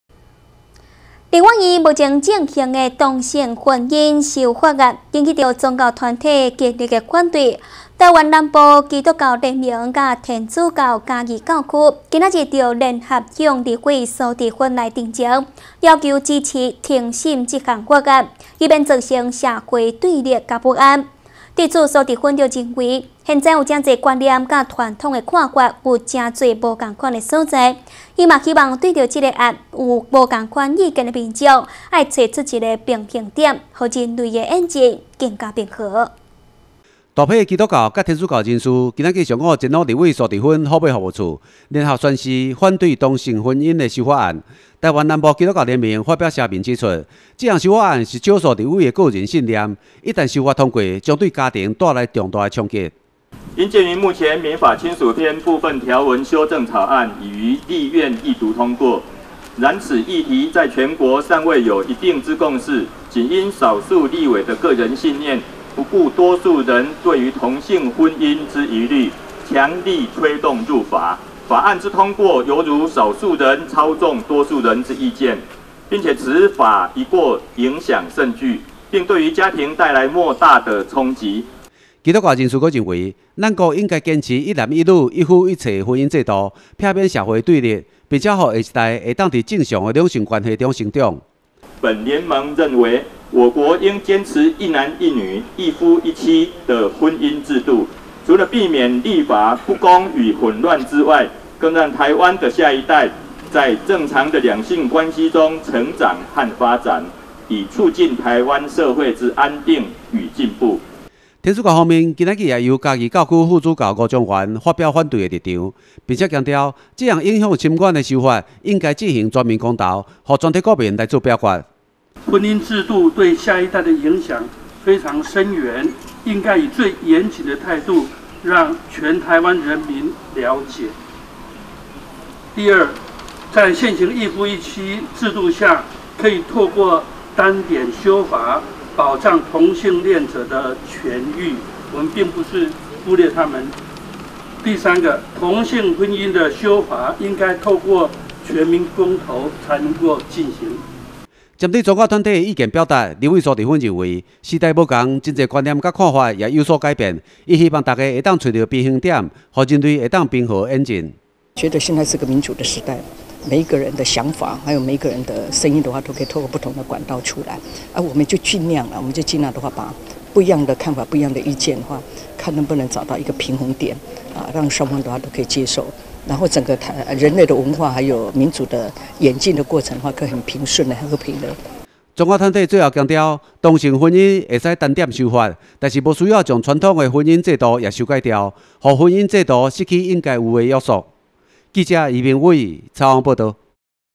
雲林新聞網─虎尾基督與天主教徒反對同性婚姻入法
立法院目前進行中的同性婚姻修正法案，引起宗教團體激烈反對；台灣南部基督教聯盟與天主教嘉義教區，今日就聯合向立法蘇治芬陳情，要求支持停審這項法案，以免造成社會的對立與不安；對此，蘇治芬認為，現在很多觀念和傳統的看法，都有很多不同的見解，她希望對本案有不同意見的民眾，要找出一個平衡點，讓人類民主的演進更加平和。〈以上新聞可從哈TV APP收視〉